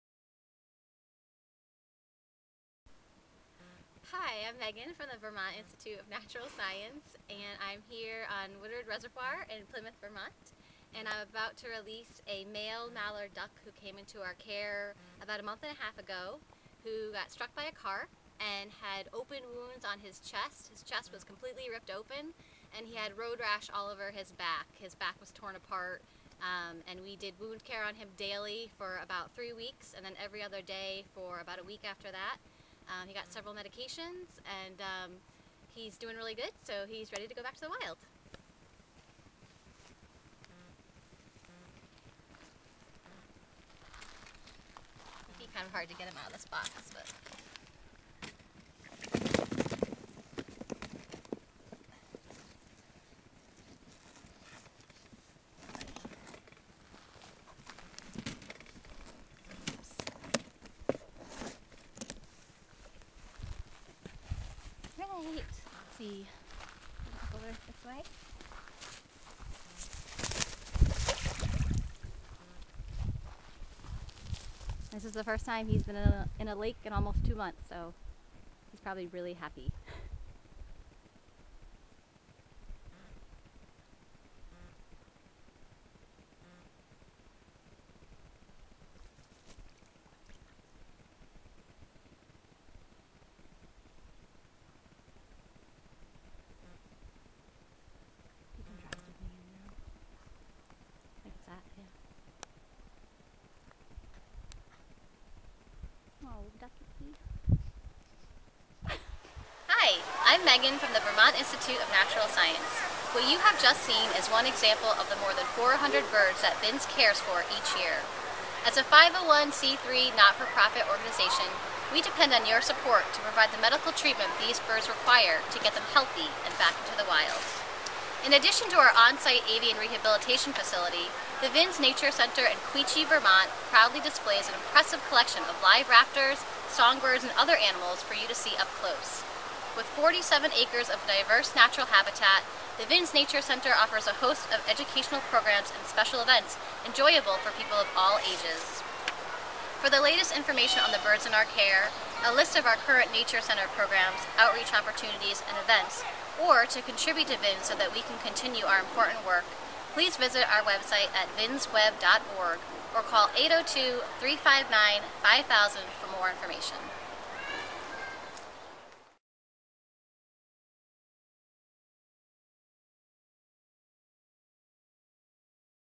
Mallard Back in the Game
This mallard duck was released back into the wild in Plymouth, VT by a Vermont Institute of Natural Science (VINS; Quechee, VT) staff member. The duck sustained severe wounds after being struck by a car and dragged across the road. In the video, Wildlife Services Manager Meghan Oliver explains the ducks' recovery. To read more about the birds in our care, check out our blog To learn more about VINS, visit our web